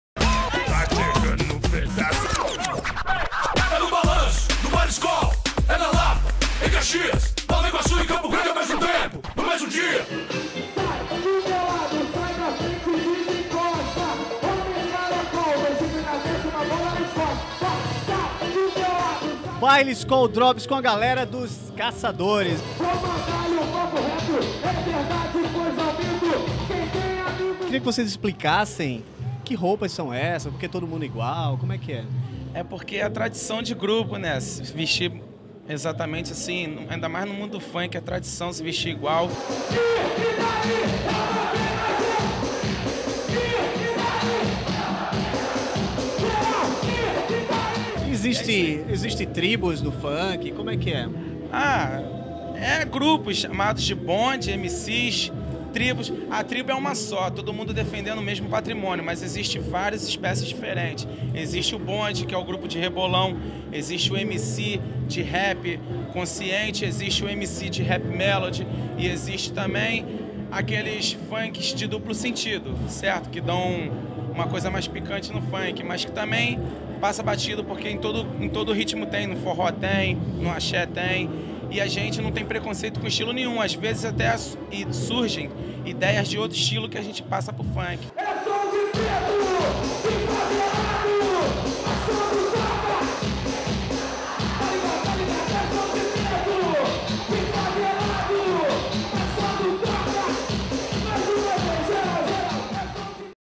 4a-Moda e Estilo1 - Baile Skol 2007 - Matéria
Direção: Patrick Korb
Evento patrocinado pela Skol, realizado em 4 locais diferentes e simultâneamente no Rio de Janeiro.